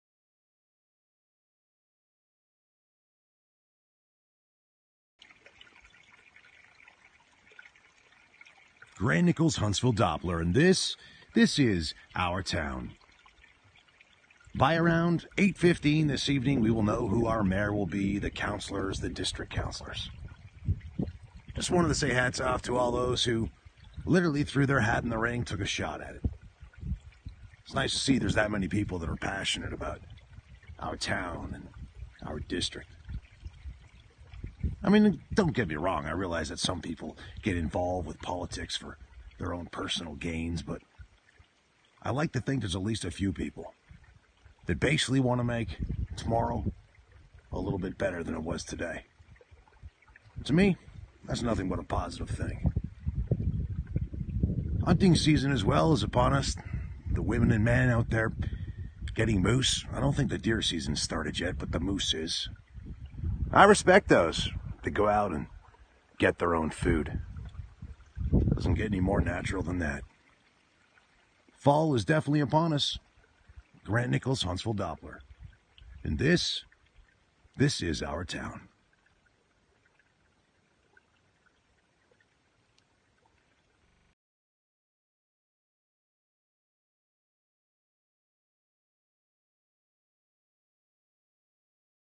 Our Town with Grant Nickalls: Election day
This week in Our Town, today is election day and by tonight we'll know who will comprise Huntsville's council for the next four years. You have until 8:00 p.m. to vote.